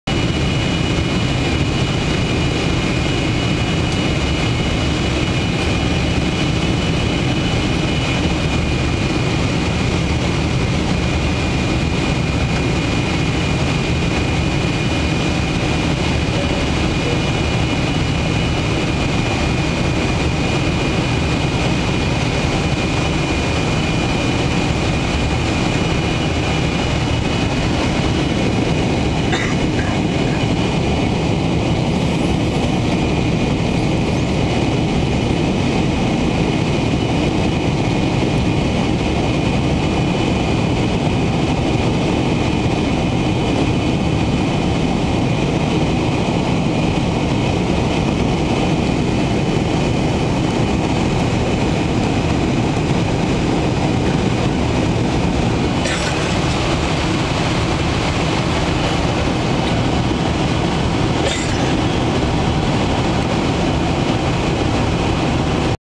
Singapore Airlines Boeing 747-400 (Flight SQ220)
A view of the Eastern Australian coastline from Seat 61K after take off from Sydney's Kingsfort-Smith International Airport (SYD). The jumbo jet, Flight No. SQ220 makes its way to Singapore's Changi International Airport (SIN) on Saturday 18th August 2007. Probably the best airline in the world!